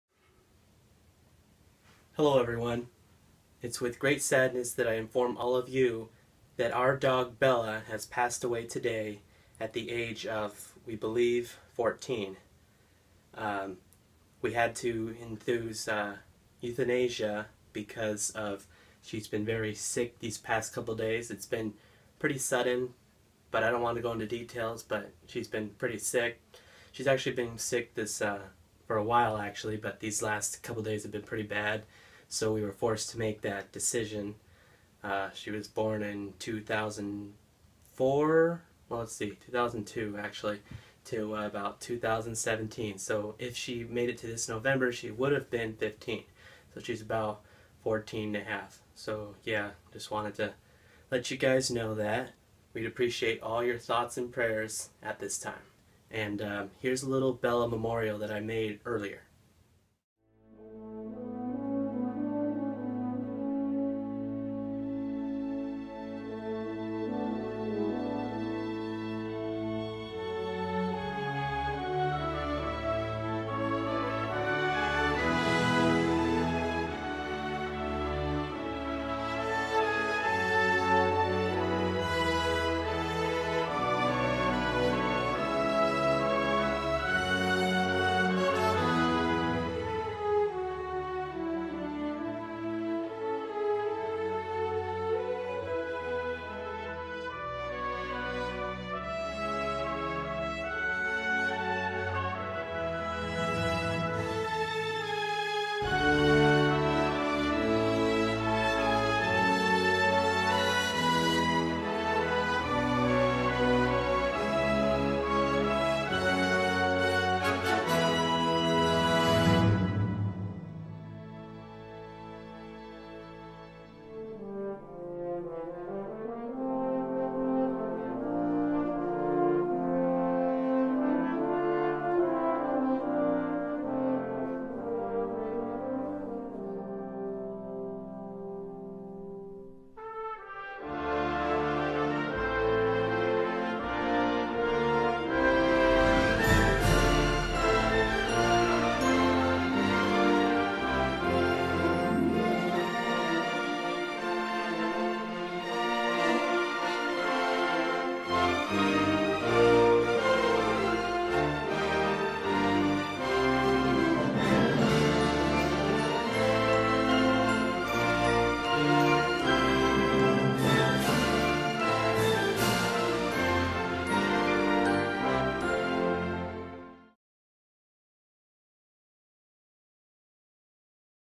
RIP our dog Bella April 2017
Our Dog bella past away today. More info In video plus memorial slide show.